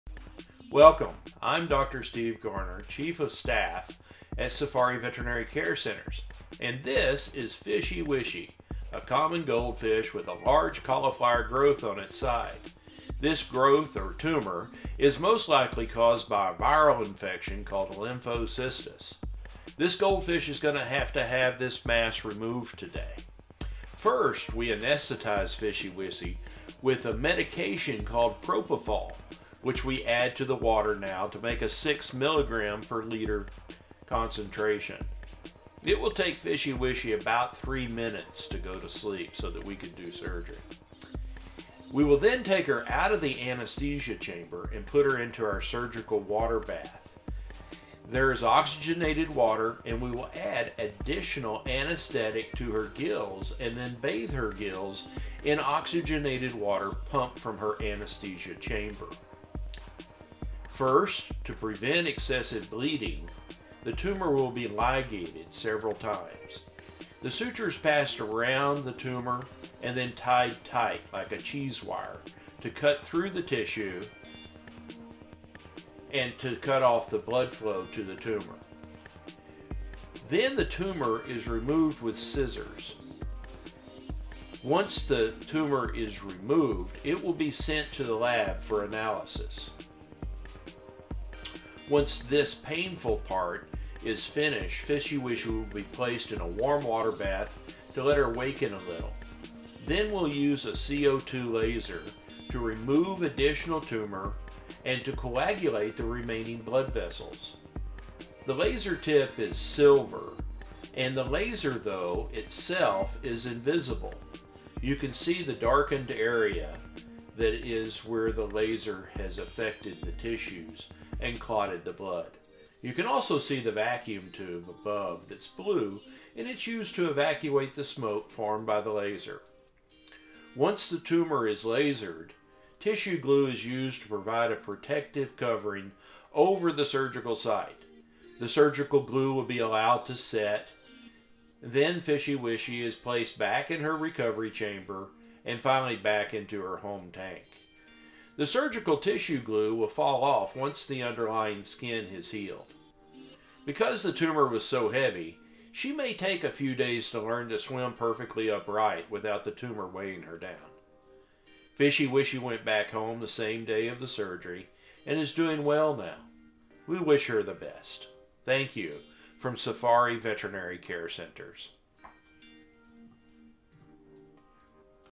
Goldfish Surgery
Fishy Wishy is a common goldfish that is undergoing surgery for tumor removal.